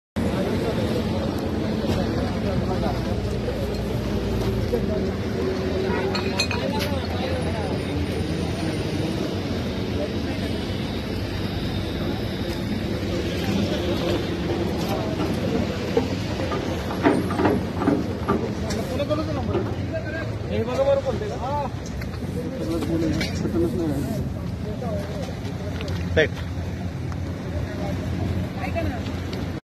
Accident On Mumbai Pune Express | 5 peoples Dead | 3 Injured| Flash Tv News
मुंबई पुणे महामार्ग पर हुई दुर्घटना

मुंबई पुणे महामार्ग पर गाड़ी की हुई दुर्घटना।

जिसमे 5 लोगो की मृत्यु और 3 लोग जख्मी बताई जा रही है।

दुर्घटना कल रात 11.30 बजे हुई।

पुलिस जगह पर पहुची और जख्मियों को अपस्टाल भेज गया।

गाड़ी नं - MH14 EC3501